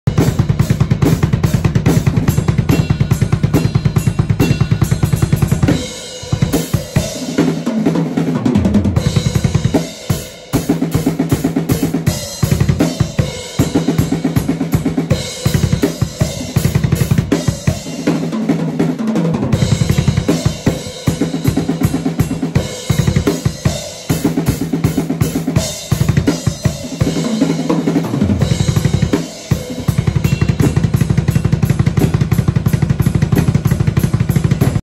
Double Kick Practice
Practicing while exhausted is a challenge, especially while trying to learn some brutal death metal. Nothing helps me improve more than watching these terrible videos of my awful technique. Here’s a tip, if you think you’re a badass at something, take a video of you doing it and watch it. If you care at all it’ll put you right back to work! Lol..